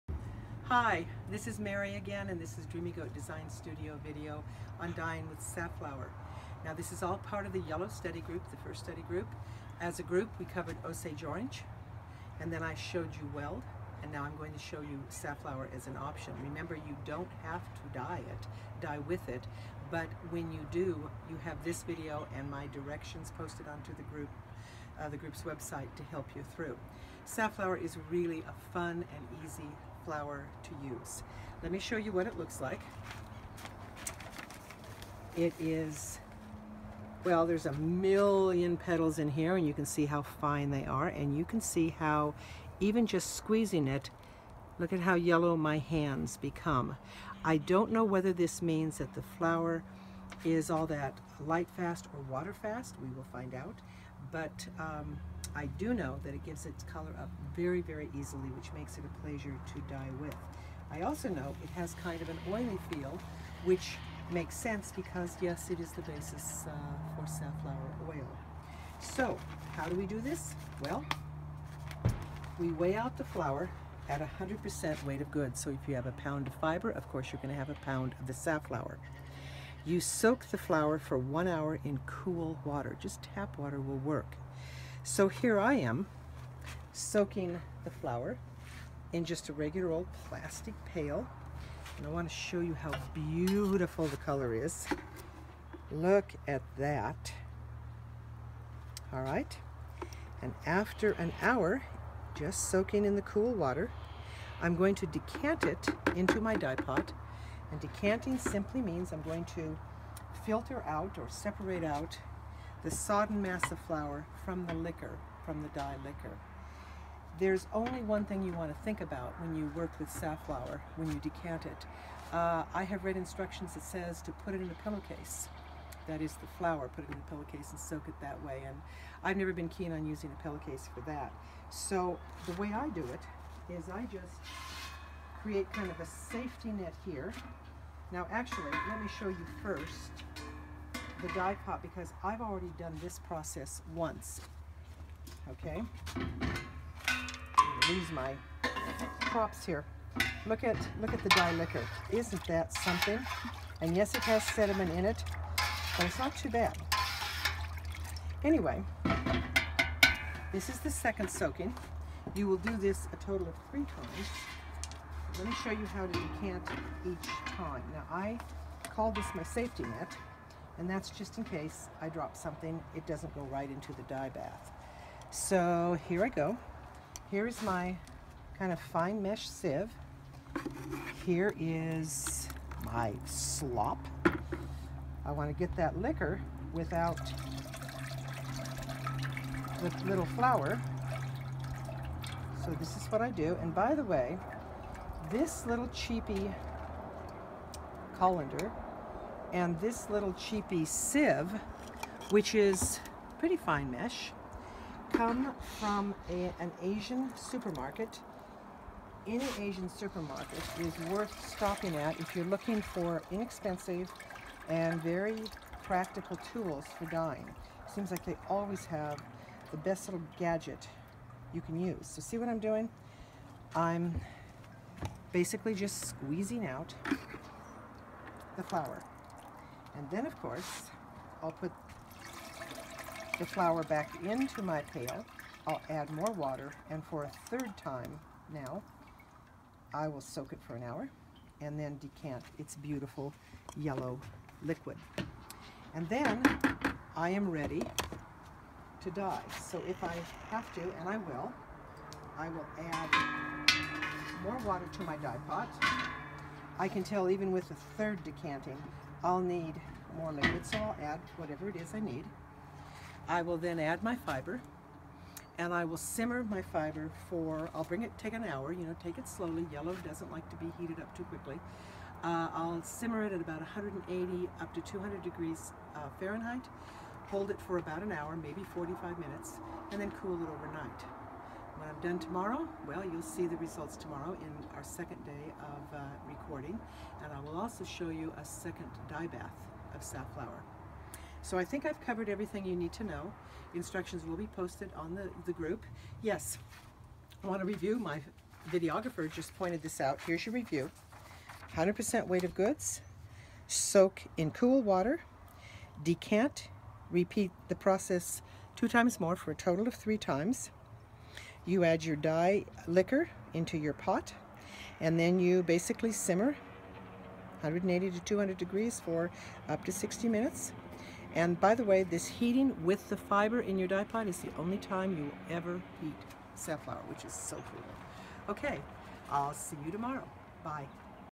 Dreamy Goat Design Studio--Dyeing with Safflower - Day 1
Safflower is an easy botanical to dye with.  This video will show you just how easy it is.